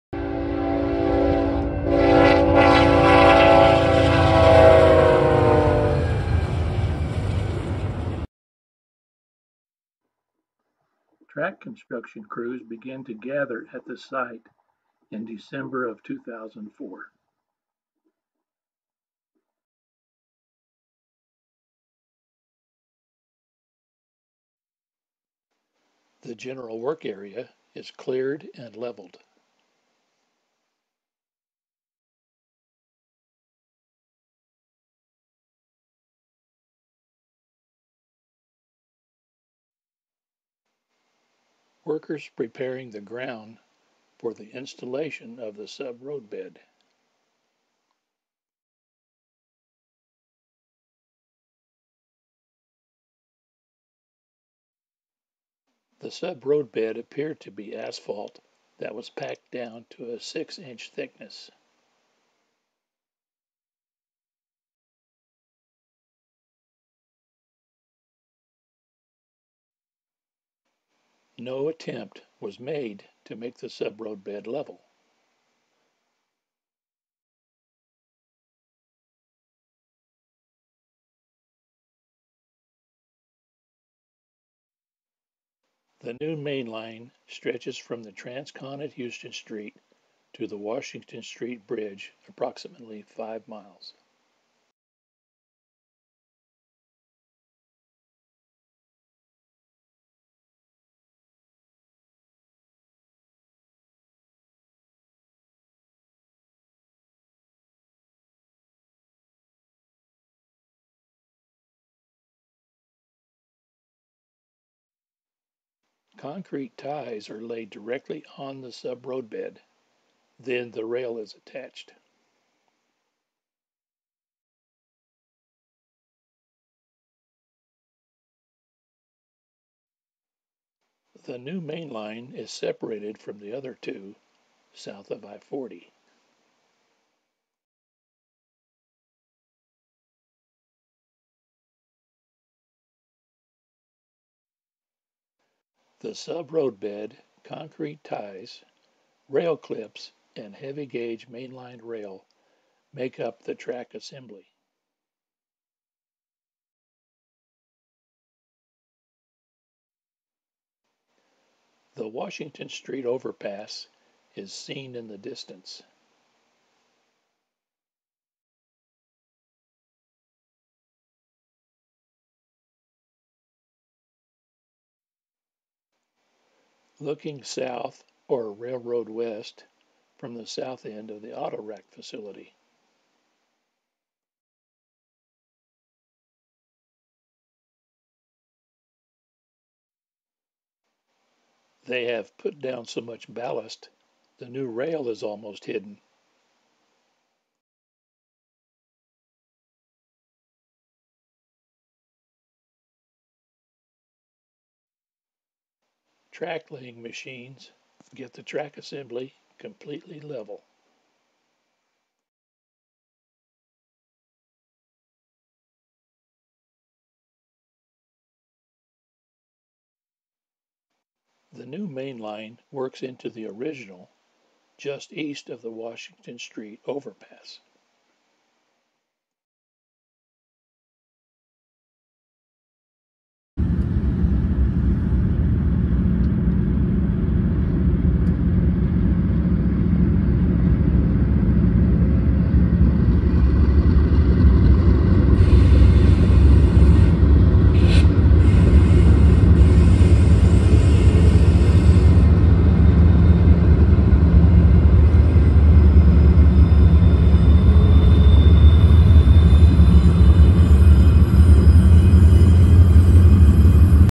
AMARILLO EXPANSION! BNSF Builds the 3rd Mainline on the Transcon (2005 Construction)
*MEGA PROJECT!* This video captures the logistics and engineering of a major railway expansion: the installation of the Third Mainline for Amarillo's famously busy BNSF Transcon in 2005.

It’s a fascinating look at the construction process, showing how crews lay a completely new mainline right alongside the two active mains of the Transcon—all while trains continue to move at speed! The shift to modern track-laying is evident, with the prominent use of concrete ties speeding up the process and improving track stability for future high-speed operations.

Watch the machinery, the manpower, and the precision required to complete this critical piece of infrastructure. This project was key to boosting capacity on one of the busiest rail corridors in the country.

*Project Details:*

* *Location:* BNSF Transcon, Amarillo, Texas

*Year:* 2005

* *Focus:* Laying of the 3rd mainline track

* *Key Feature:* Modern track laying with concrete ties